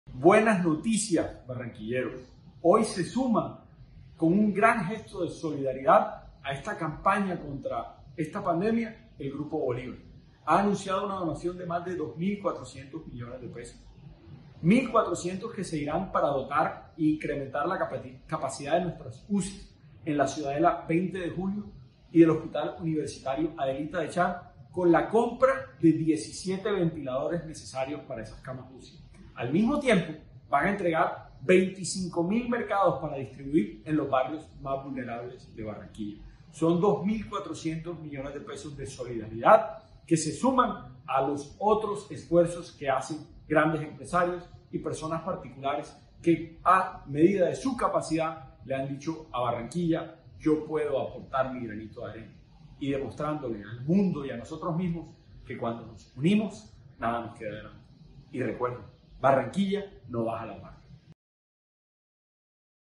Alcalde Pumarejo agradece donaciones de Grupo Bolívar para atender crisis de coronavirus
Grupo Bolívar se suma a esta campaña contra el COVID-19, con un apoyo solidario por más de $2.400 millones. Es la mayor donación que recibe la ciudad hasta la fecha. Ventiladores, elementos de protección para personal de salud y auxilios alimentarios hacen parte de este aporte.